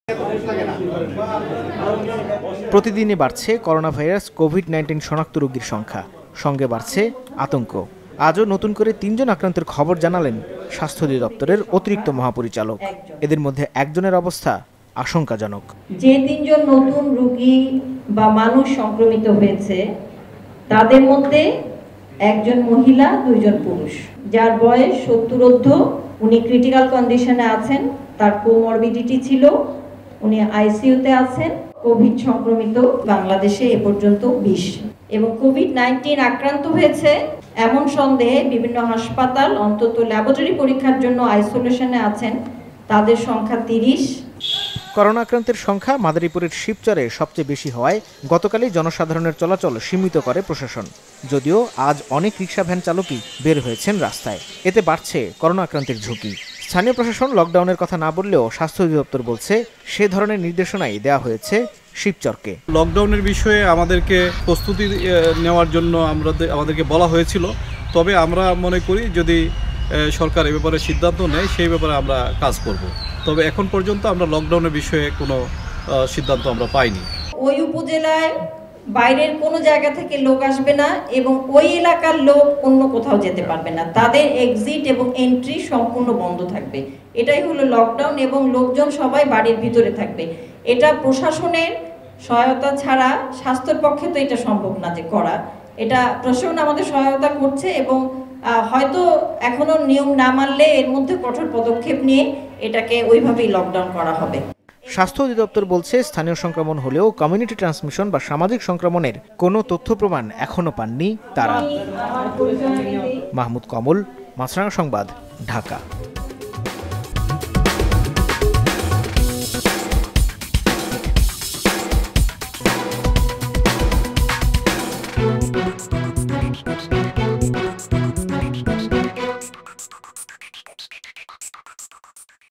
নতুন করে দেশে করোনা ভাইরাস কোভিড নাইনটিনে আক্রান্ত হয়েছেন আরও তিনজন।
নতুন করে দেশে করোনা ভাইরাস কোভিড নাইনটিনে আক্রান্ত হয়েছেন আরও তিনজন। এনিয়ে মোট আক্রান্তের সংখ্যা বিশ। নতুন আক্রান্ত তিনজনের একজনের অবস্থা আশঙ্কাজনক।